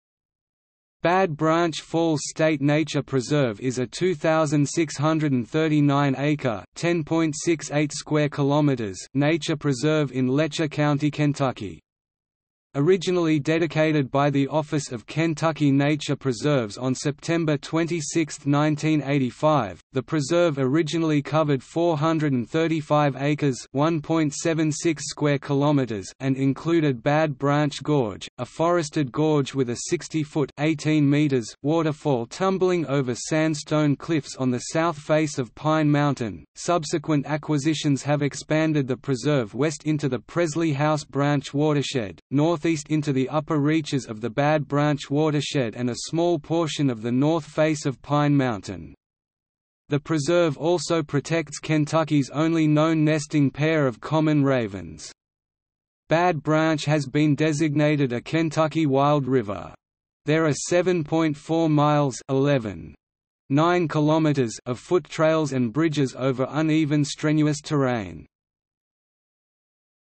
Bad Branch Falls State Nature Preserve | Wikipedia audio article
- Socrates

SUMMARY
Bad Branch Falls State Nature Preserve is a 2,639-acre (10.68 km2) nature preserve in Letcher County, Kentucky. Originally dedicated by the Office of Kentucky Nature Preserves on September 26, 1985, the preserve originally covered 435 acres (1.76 km2) and included Bad Branch Gorge, a forested gorge with a 60-foot (18 m) waterfall tumbling over sandstone cliffs on the south face of Pine Mountain. Subsequent acquisitions have expanded the preserve west into the Presley House Branch watershed, northeast into the upper reaches of the Bad Branch watershed and a small portion of the north face of Pine Mountain. The preserve also protects Kentucky's only known nesting pair of common ravens. Bad Branch has been designated a Kentucky Wild River. There are 7.4 miles (11.9 km) of foot trails and bridges over uneven strenuous terrain.